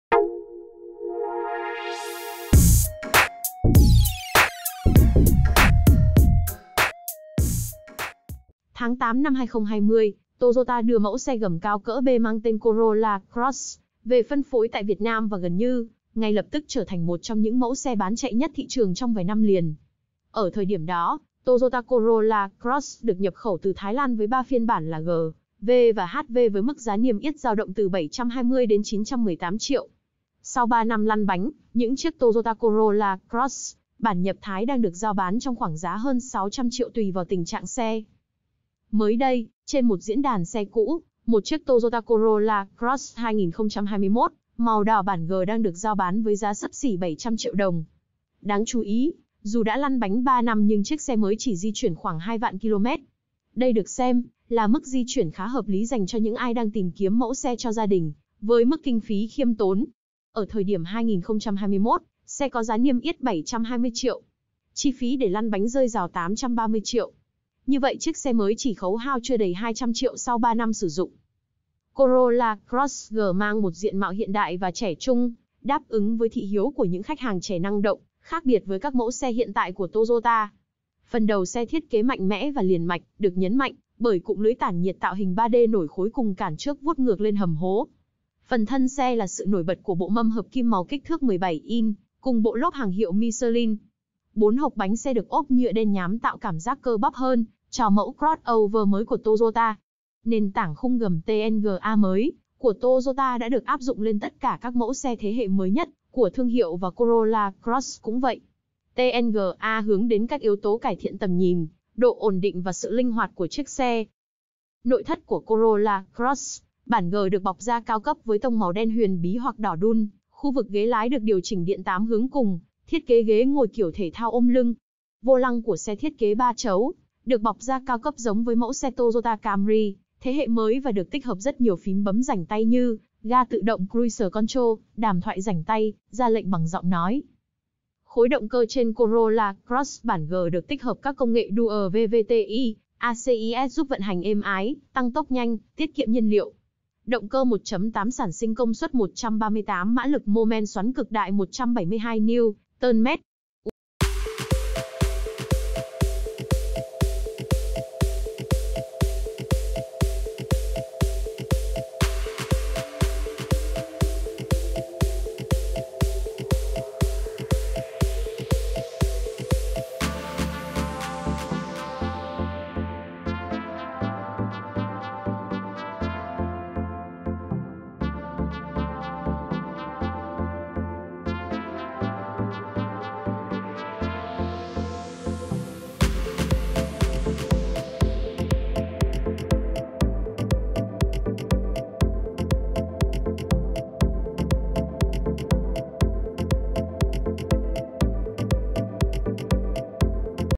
Giá lăn bánh chưa đầy 700 triệu, SUV cỡ B Toyota có giá bán rẻ đến 200 triệu sau khi chạy 20.000km
Sau 3 năm sử dụng, chiếc Toyota Corolla Cross bản 1.8G đời 2021 đang có giá bán gần 700 triệu đồng trên thị trường xe cũ.